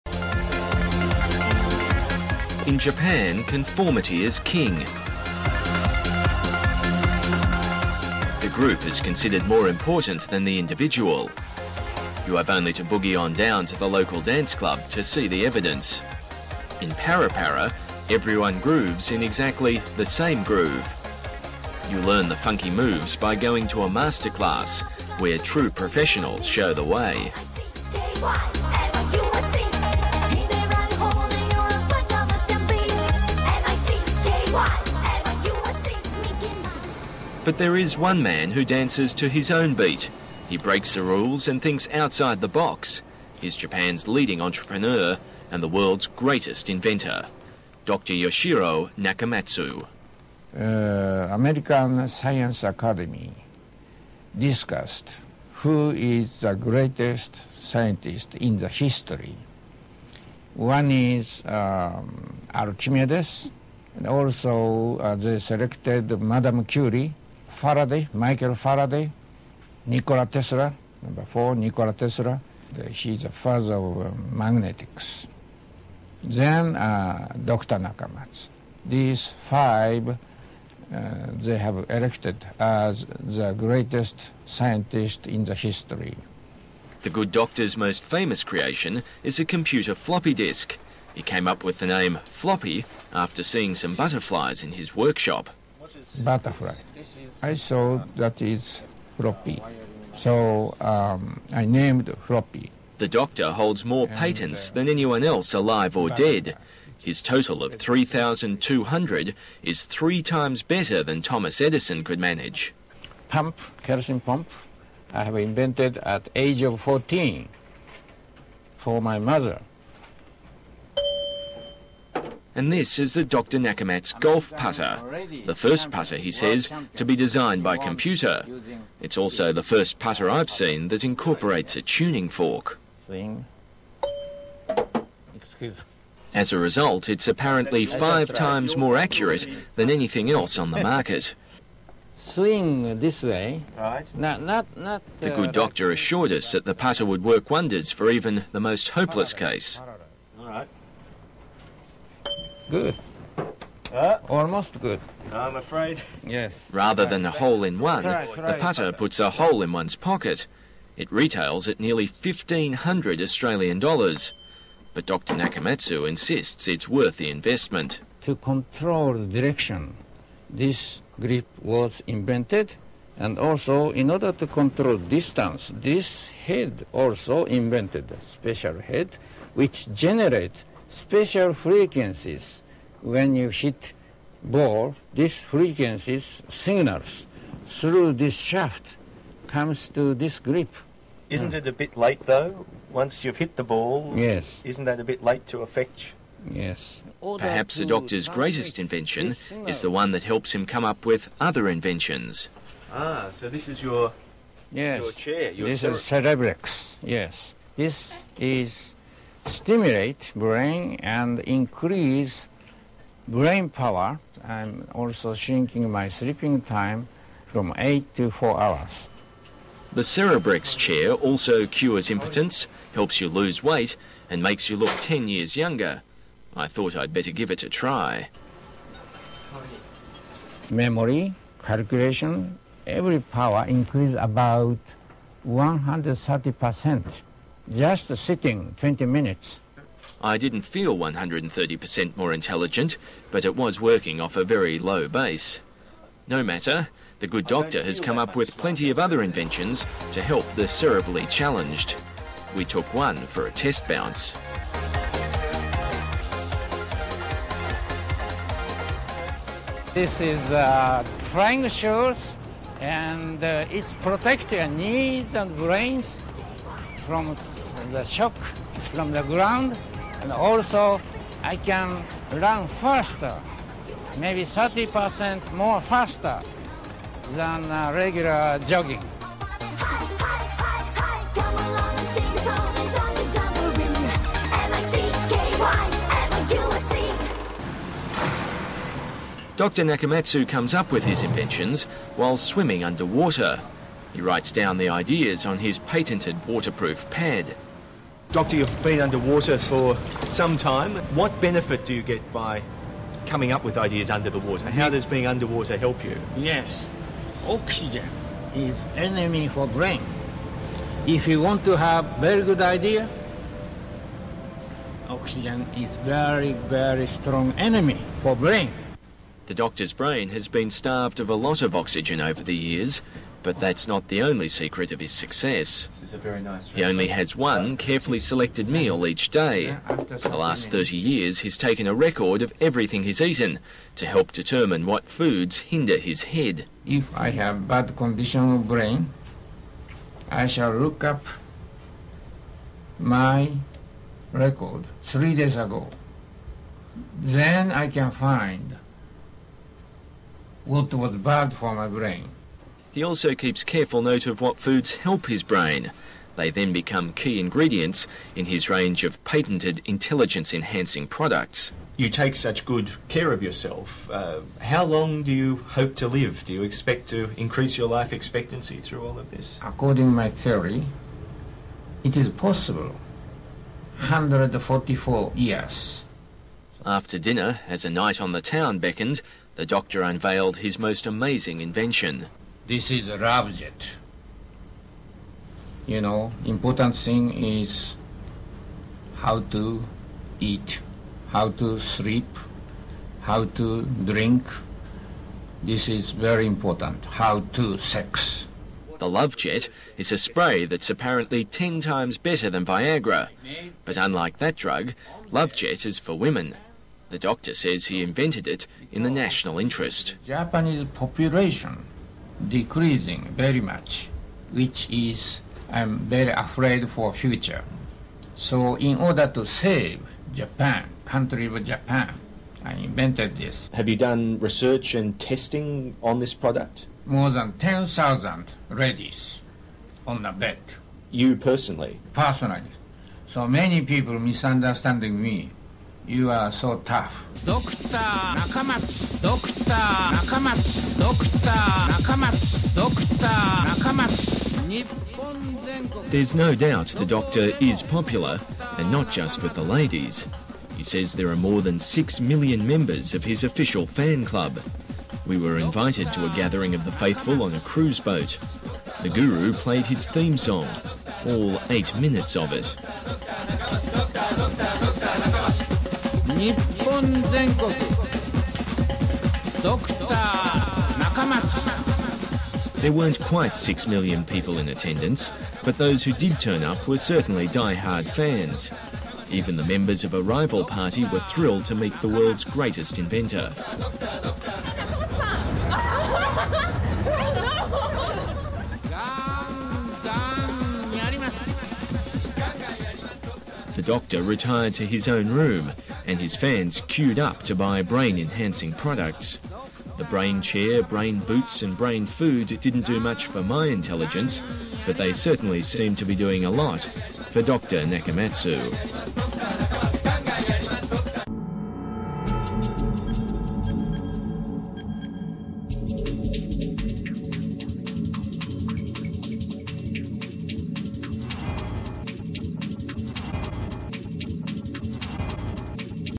Wacky Inventor - Japan
10 October 2001
Dr Nakamatsu claims to be the world's most prolific inventor

Produced by ABC Australia
Distributed by Journeyman Pictures